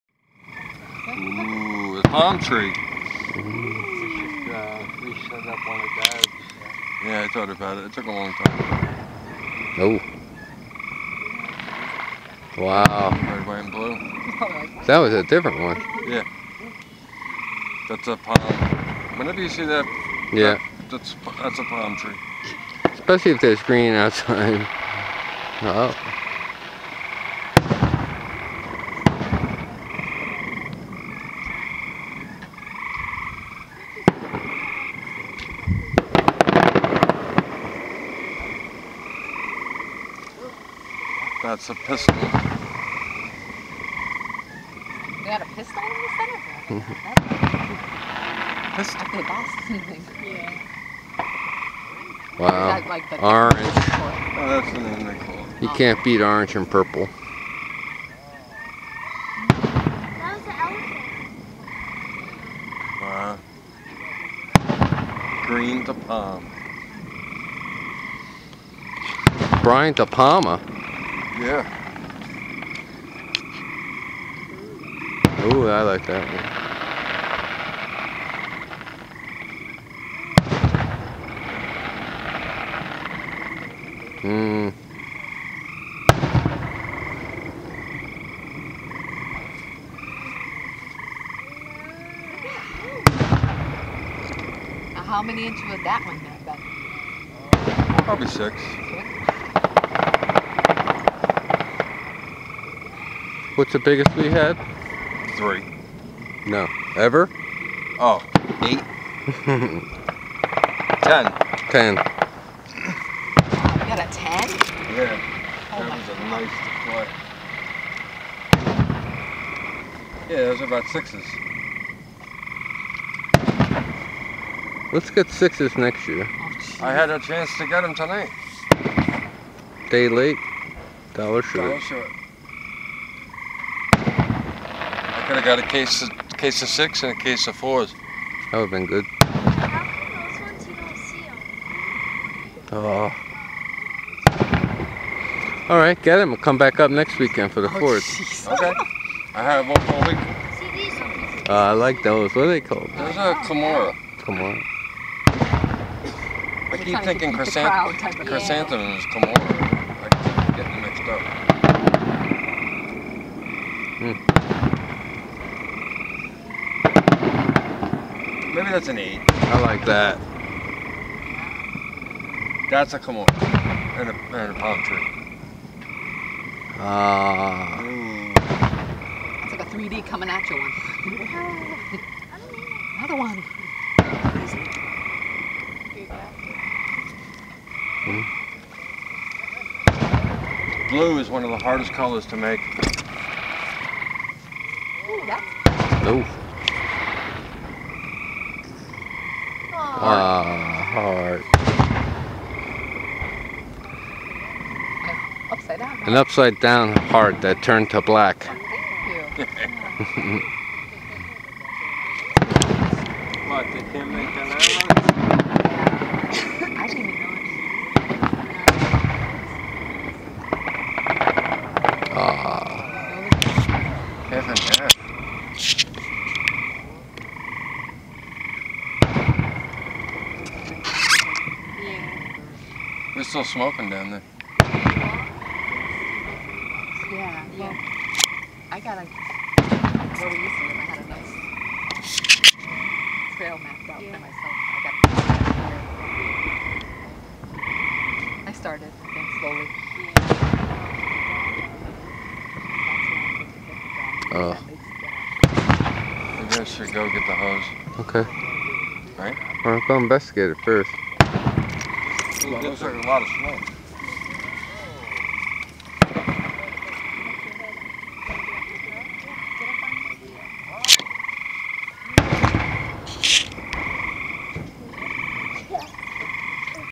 Fireworks 2013 - by the City of Easthampton, MA - number 3 of 3 - #13
City of Easthampton's firework show - 3 of 3.
Viewed/recorded from my property, 1/2 mile away
Length - 4:35

Fireworks 2013   June 29th 013